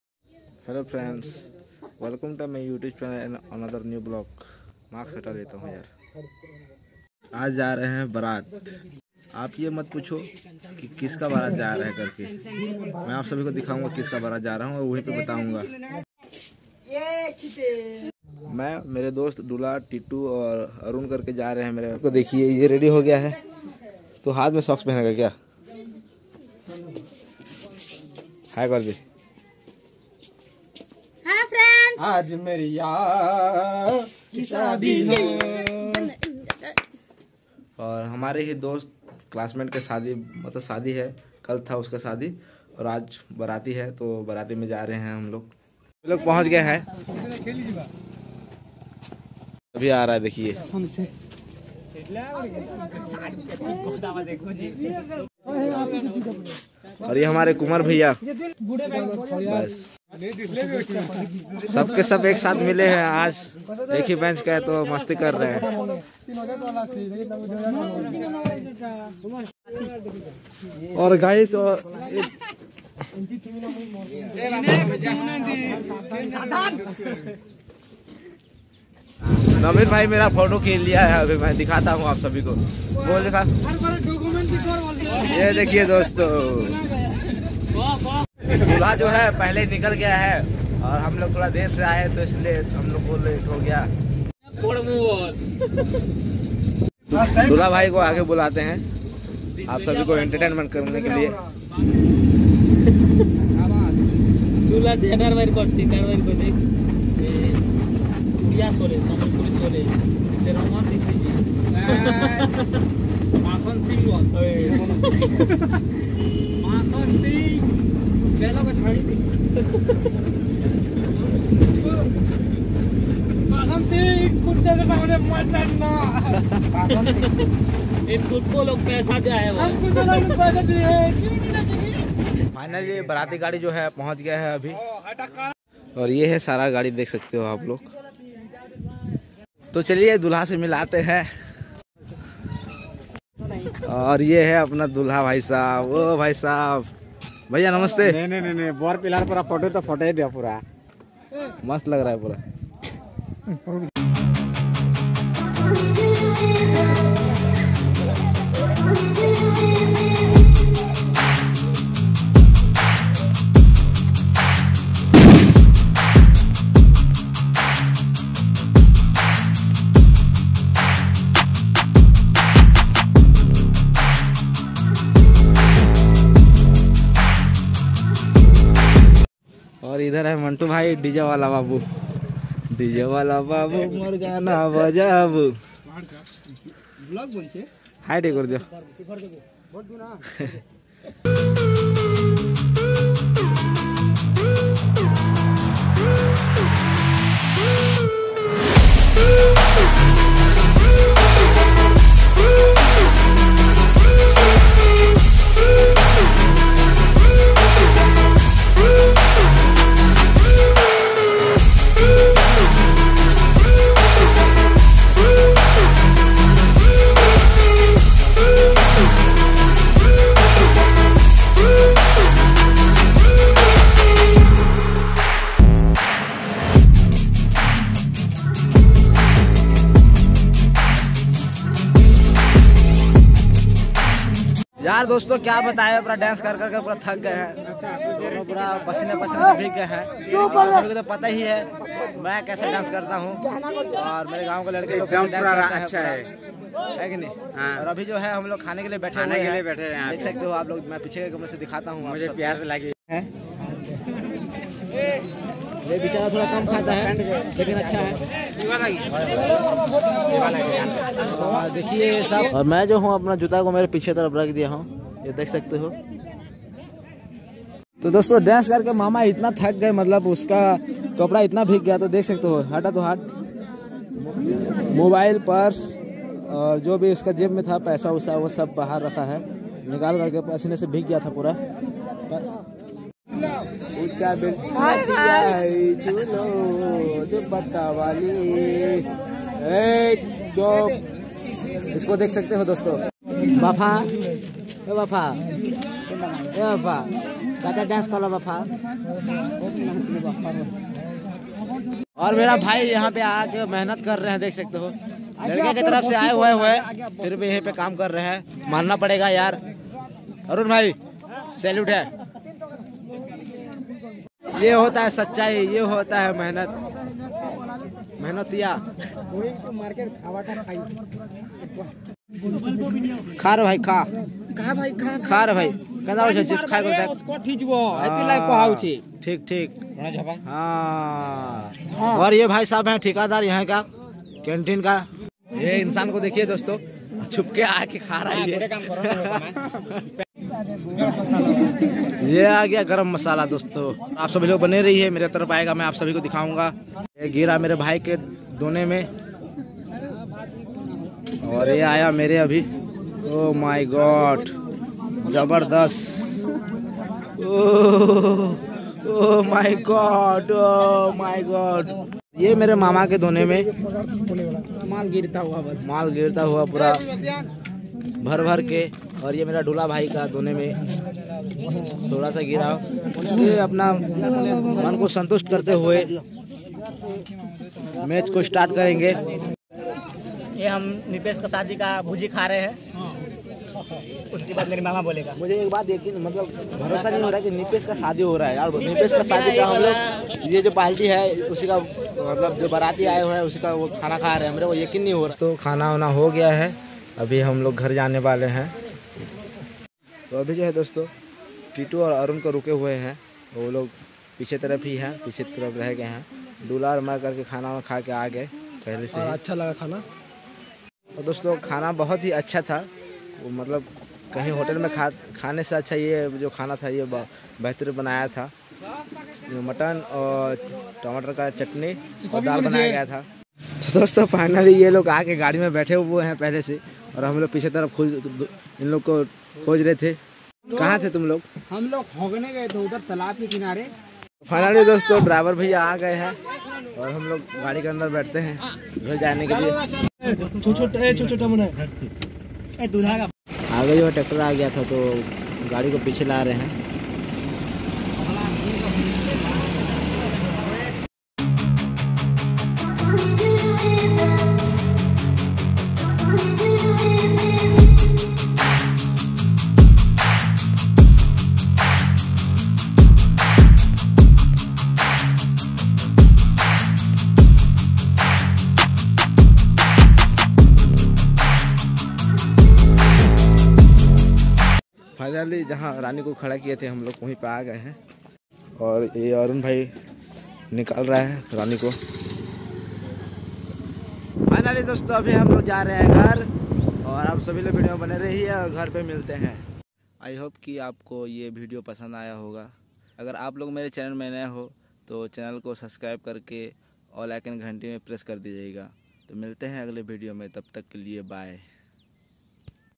Nipesh Bhai Ke Barat Me Khub dance Kiye#baratidance#kartikchhatriavlog#merragevlog#frindsvlog
Hello dosto fir se app sabhi ko swagat hai. mere channel me.ajj nipesh bhai ka  baarati hai. or hum charo jare hai. dula titu arun Or me.  dula  bhai ne  baarti me  nagin dance kiya. I hope ki app sabhi ko ye  video pasandh ayega..

aj mera yaar ki sadi hai